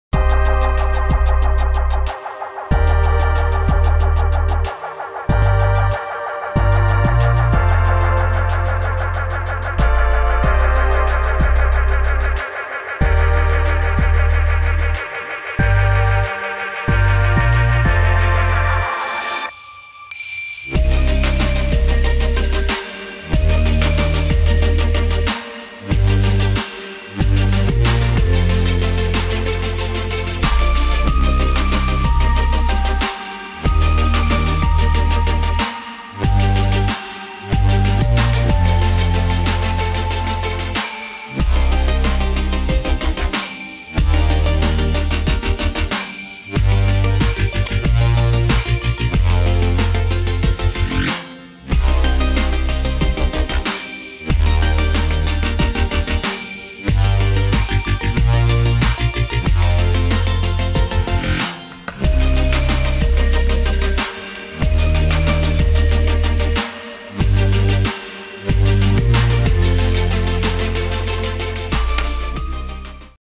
Chiba Thermofleece Touch Kerékpáros Kesztyű
Szélálló, jól lélegző, belül kifejezetten puha, fekete színű téli thermo biciklis kesztyű.